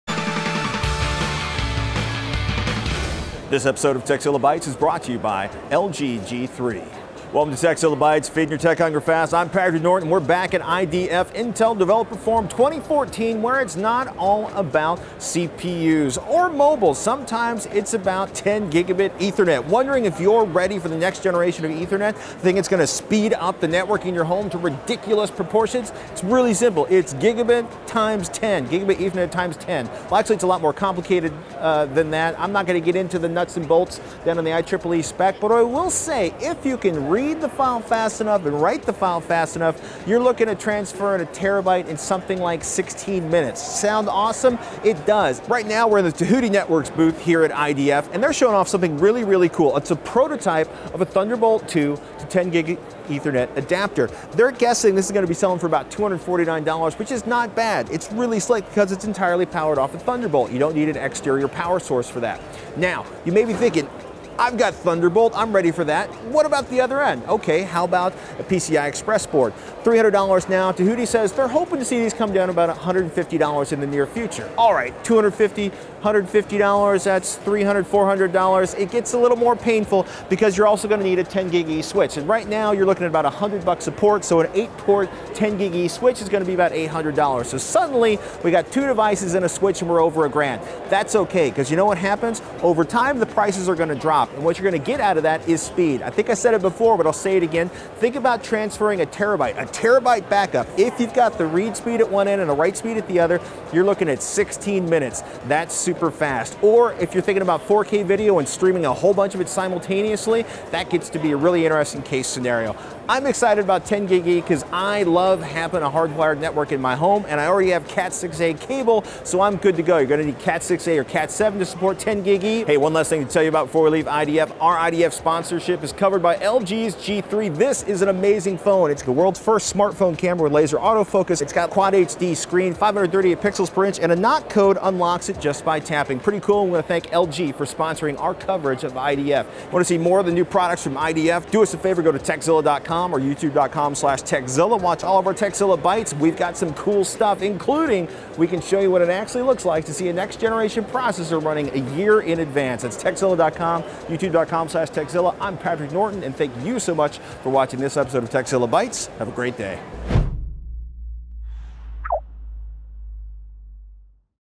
Consumer 10-Gigabit Ethernet is On the Horizon
We stopped by the Tehuti Networks booth at IDF to see a Thunderbolt 2 to 10GbE adapter. Faster ethernet speeds are coming!